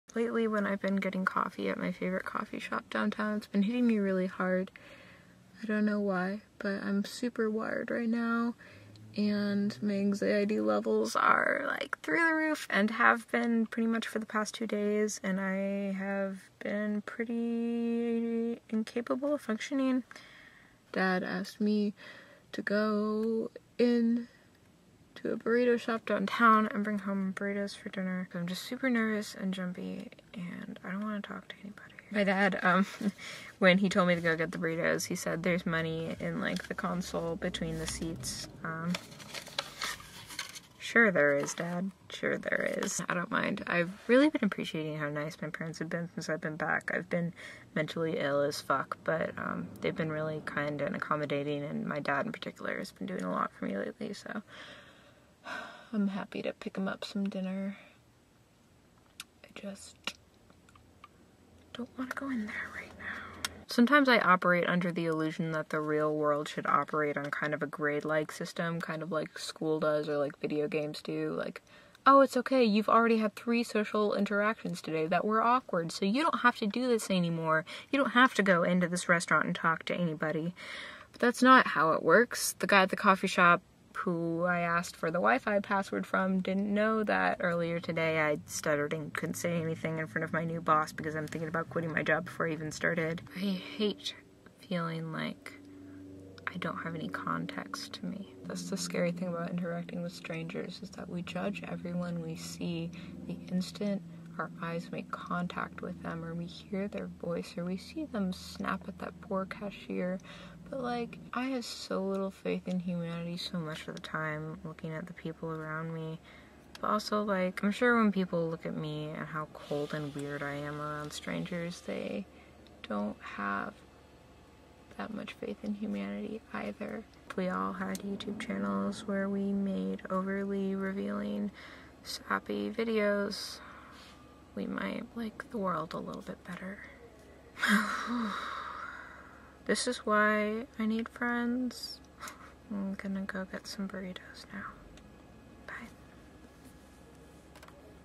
Pre-Burrito
you know when social anxiety hits you like a wet mop and you have to sit in the car for a few minutes and psych yourself up to go in to a store or something? yerp